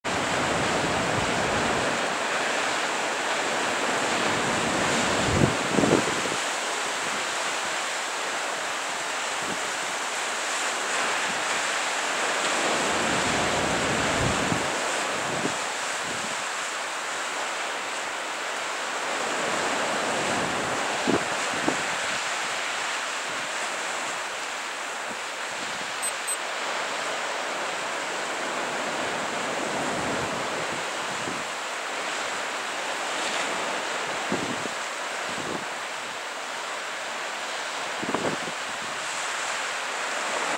白浜町の地磯、コミゾレ
北西風が吹いても、この場所だけは竿が出せます。
磯も広いので人気がある地磯の一つ。